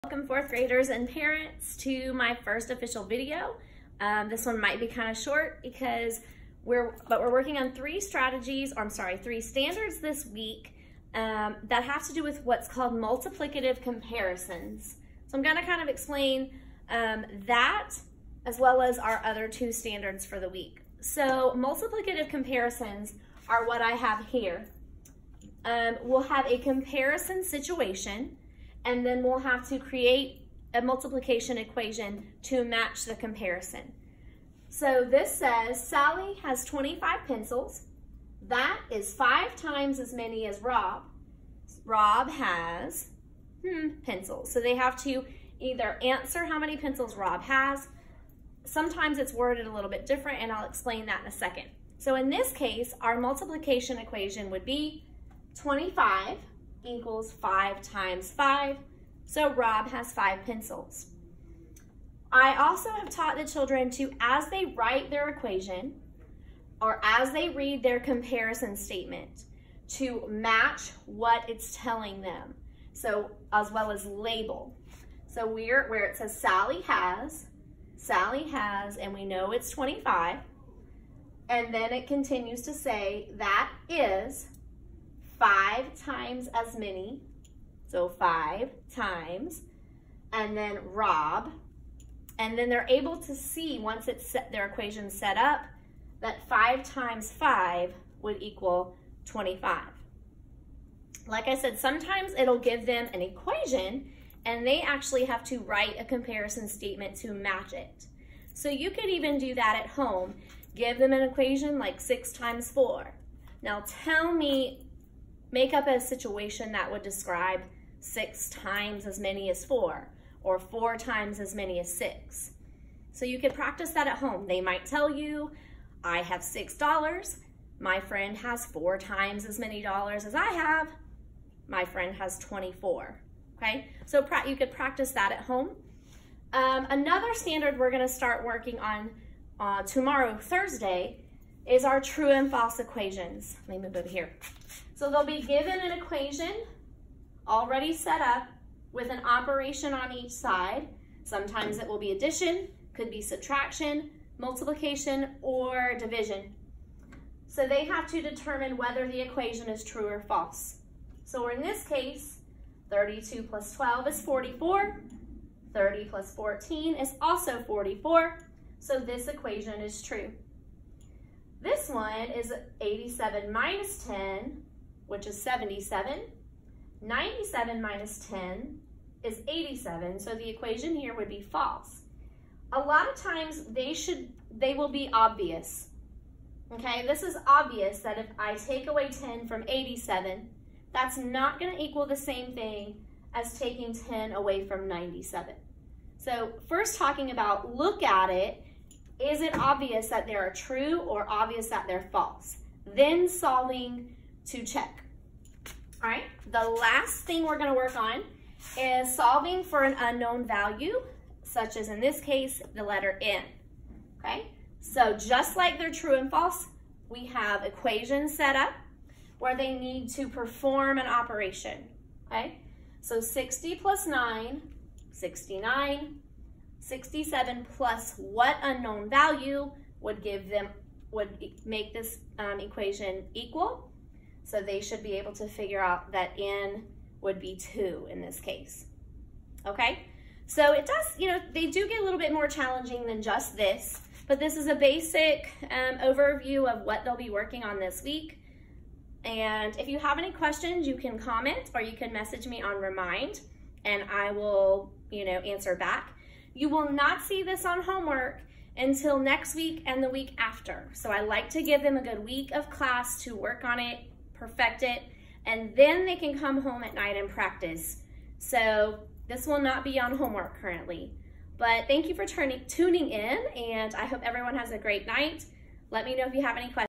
Multiplicative comparison, true or false equations, unknown values
Multiplicative Comparisons, true or false equations, unknown values. Week of September 16-20.